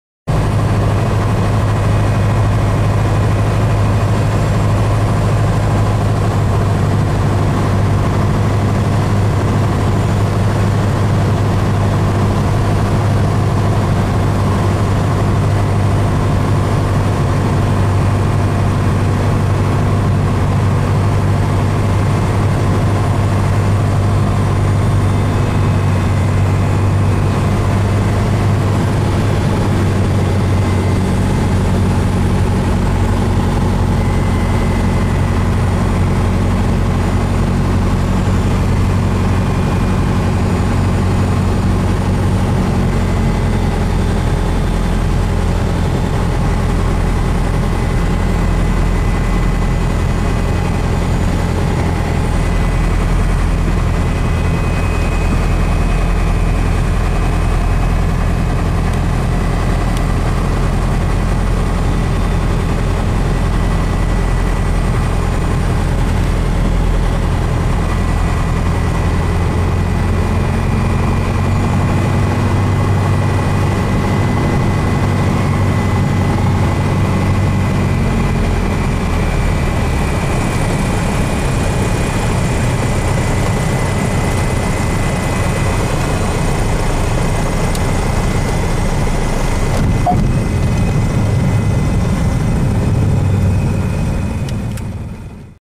Landing Runway 03 Biggin Hill EGKB
Landing on Runway 03 at Biggin Hill (EGKB) in England